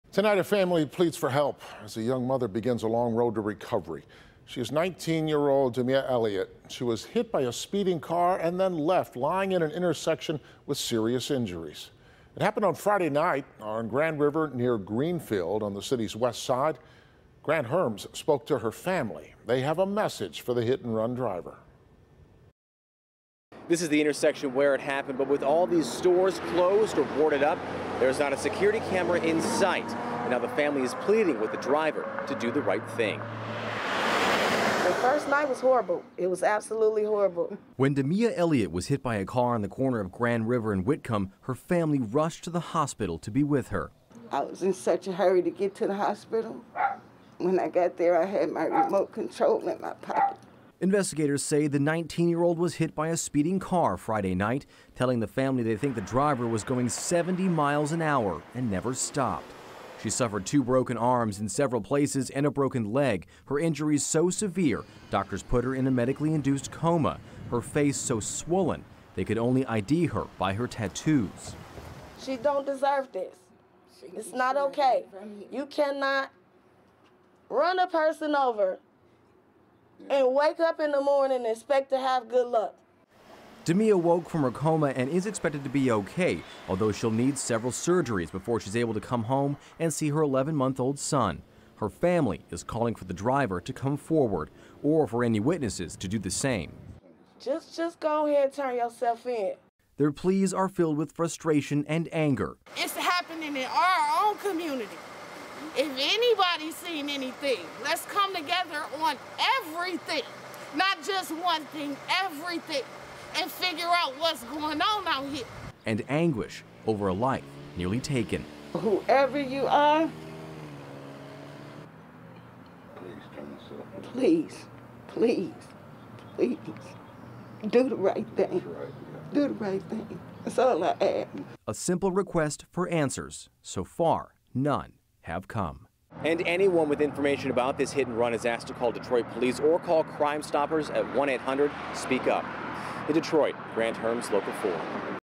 Young mother seriously injured in hit-and-run on Detroit's west side
A 19-year-old was put into a coma Friday night after a violent hit-and-run on Grand River Avenue.